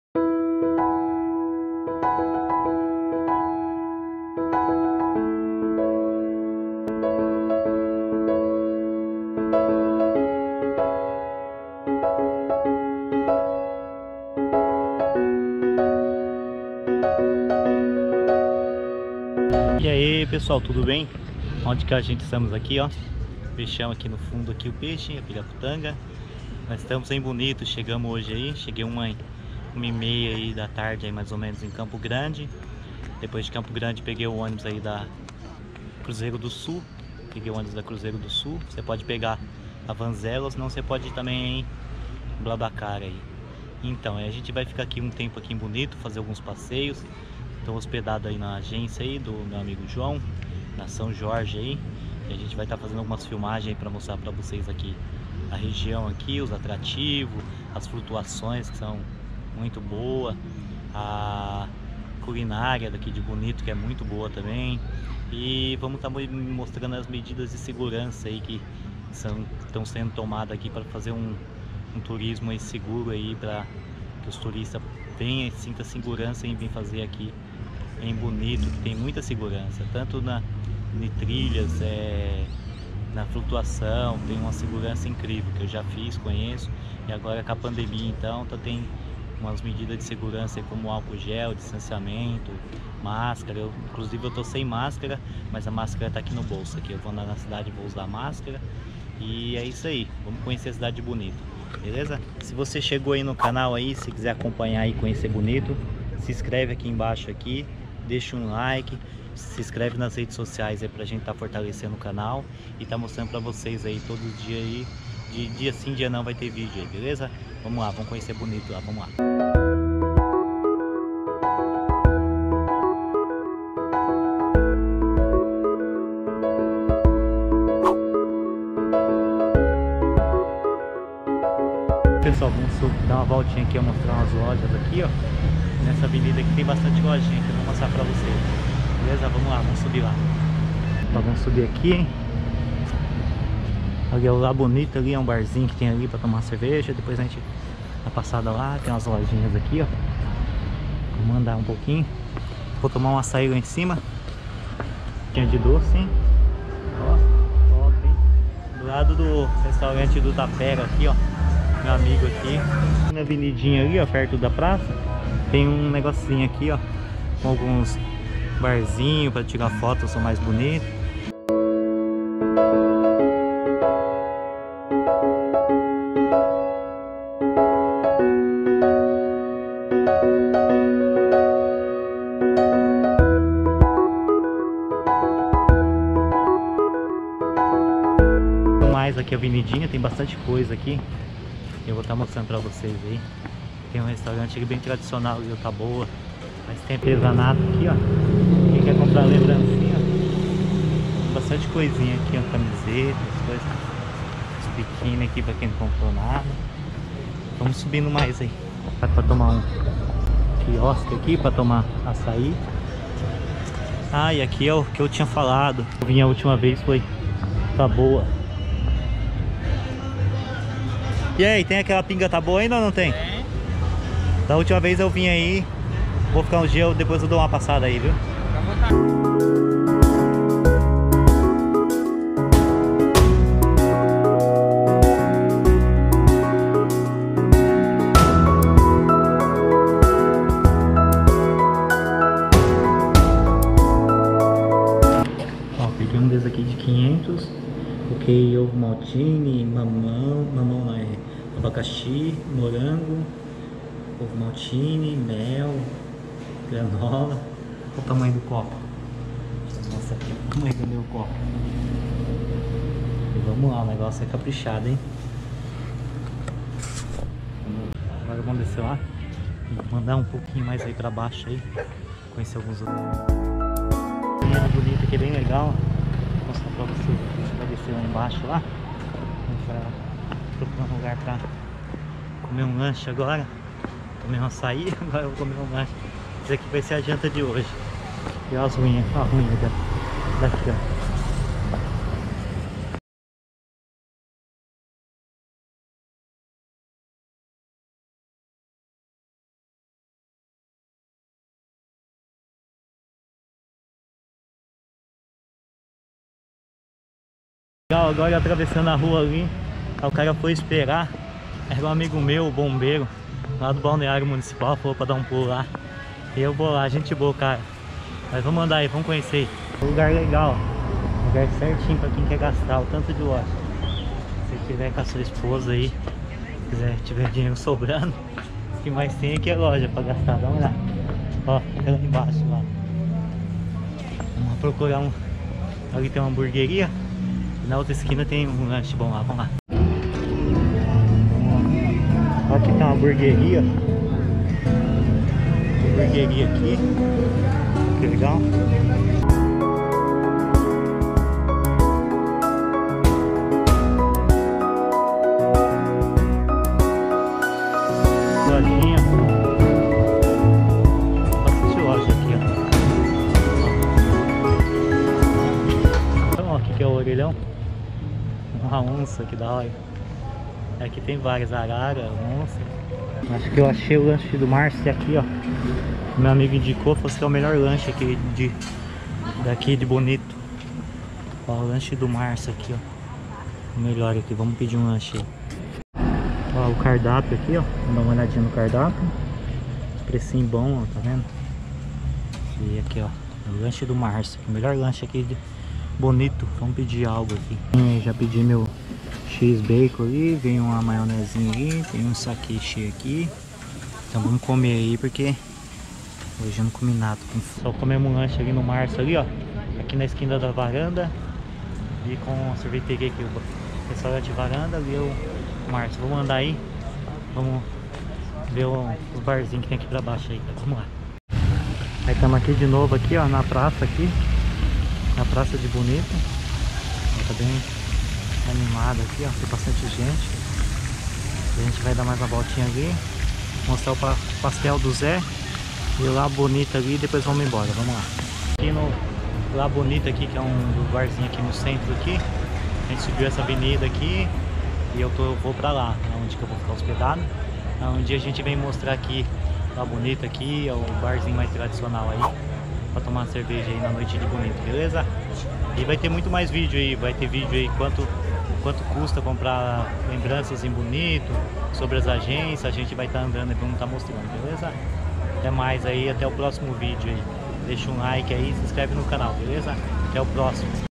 Conheça cidade de Bonito MS| Viagem na pandemia e onde Comer barato
Conheça Cidade de Bonito MS Viagem na pandemia e onde Comer barato cheguei na cidade de Bonito por Campo Grande de Ônibus e vou mostrar a você a cidade na pandemia e onde comer barato um lanche e um açaí barato andei um pouco pela cidade a noite também...
.você pode pegar um ônibus na barra funda a Campo Grande e outro ônibus de Campo Grande ate bonito (auto viação cruzeiro do sul)
ou voo direto vira copos Campinas